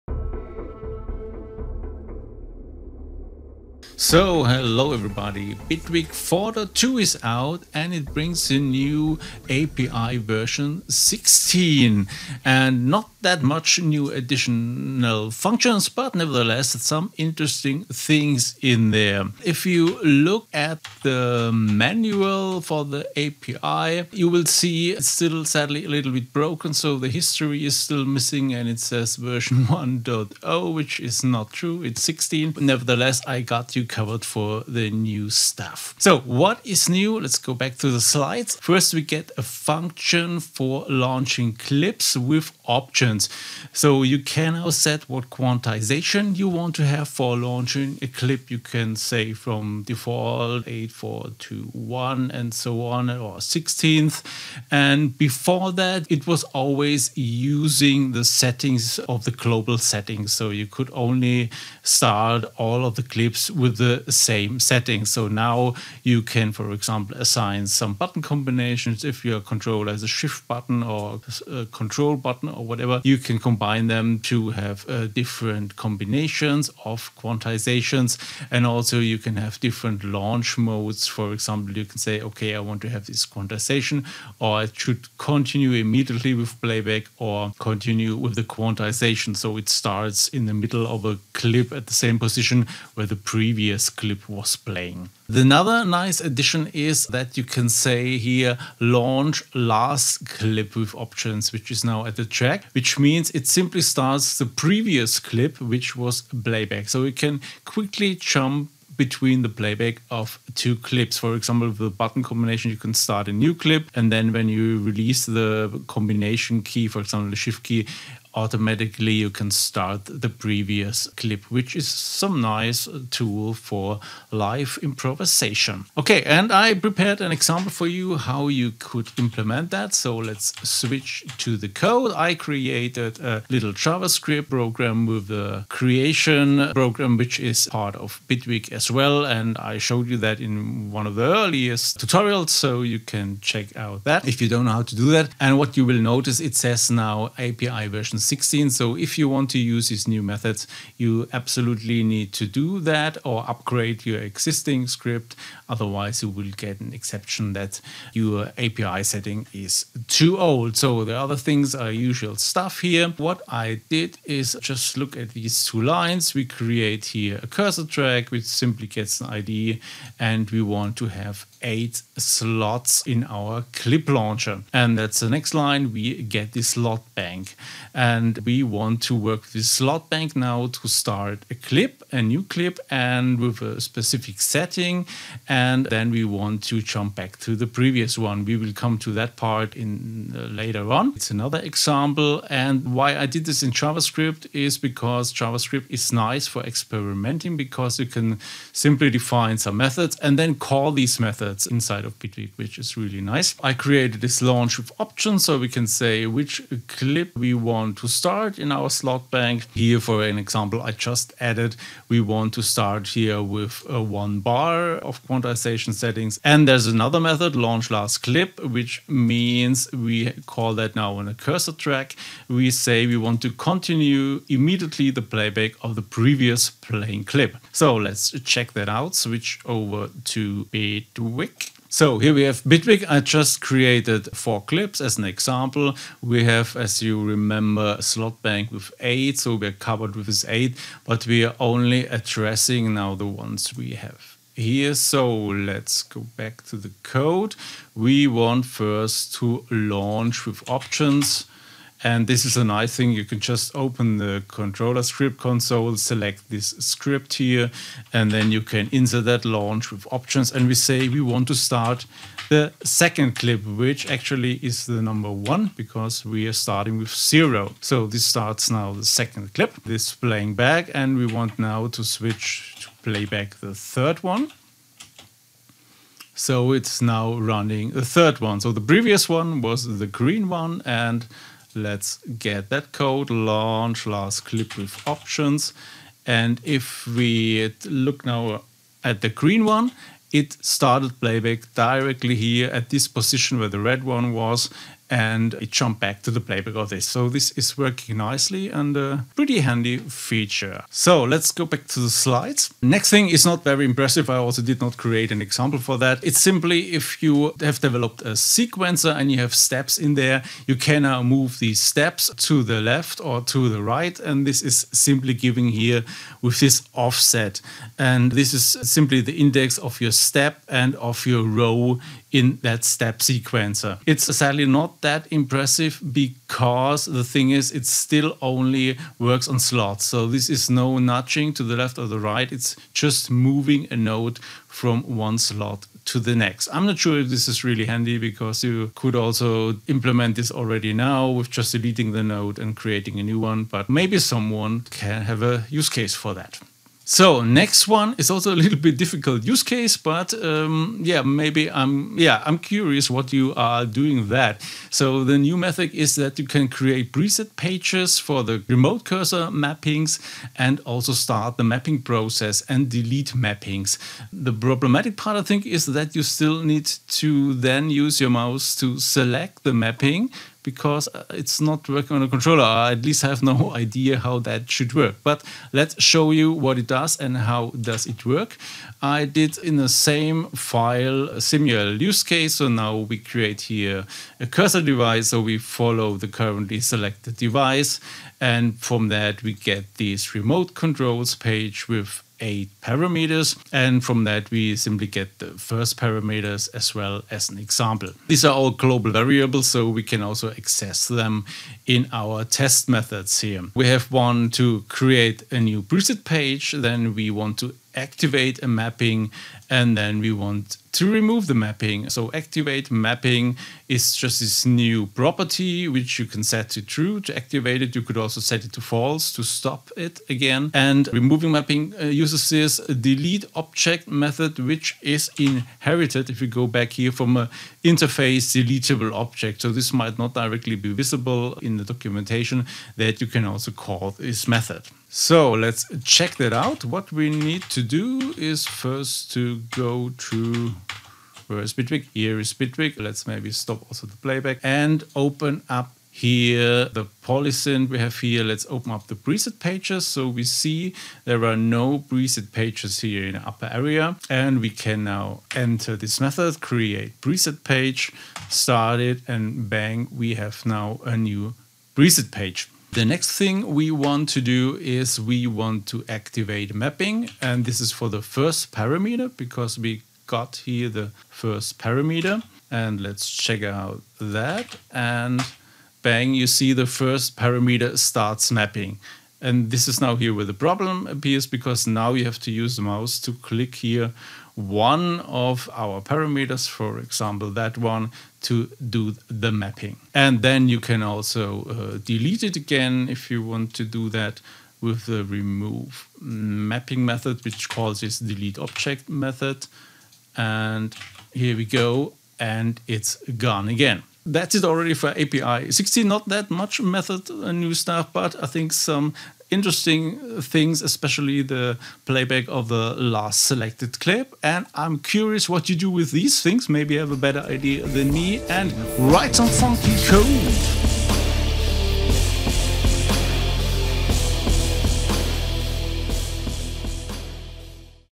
Bitwig API 16
This tutorial explains the new available methods of the Bitwig API 16 introduced in Bitwig Studio 4.2. Get the example source code